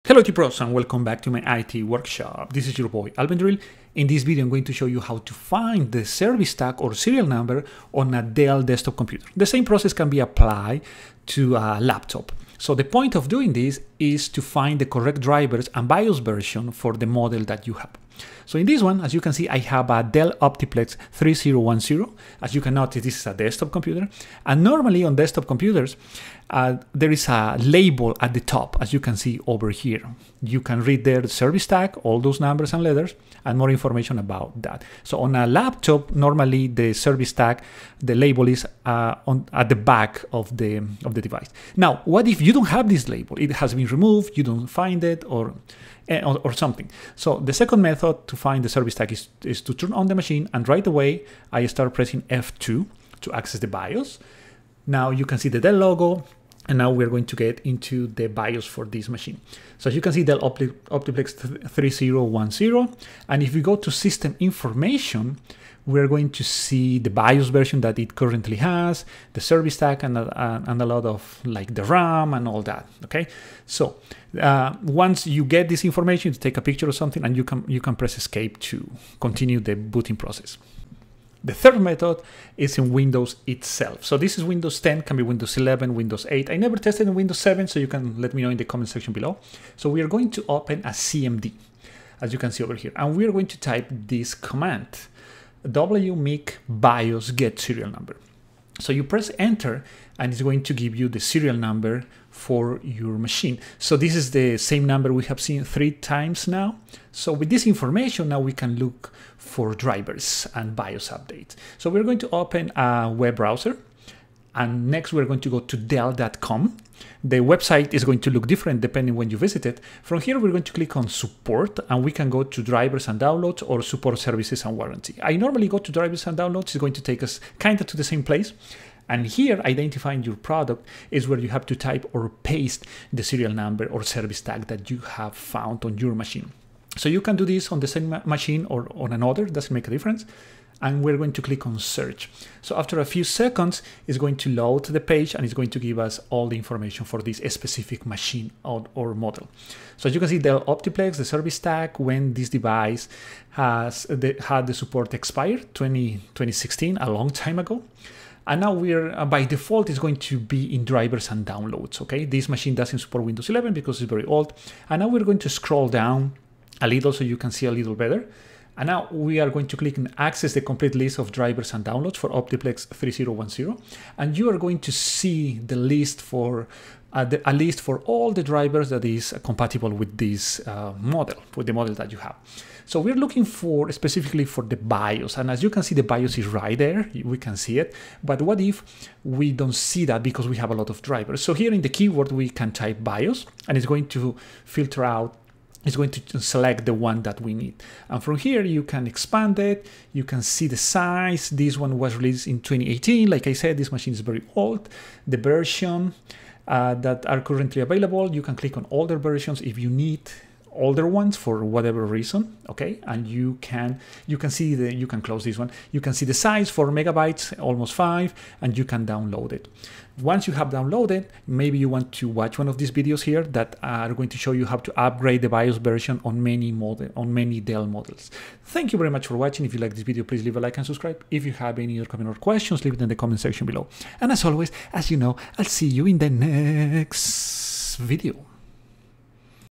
3 Ways to Find you Dell Service Tag
Hello IT Pros, this is Alvendril! In this Video, "3 Ways to Find you Dell Service Tag", I show how to find the Service Tag or Serial Number in order to download the correct Bios and drivers version for Dell device.

📌Have some time to become better? Here're some Books I like:
Rendezvous with Rama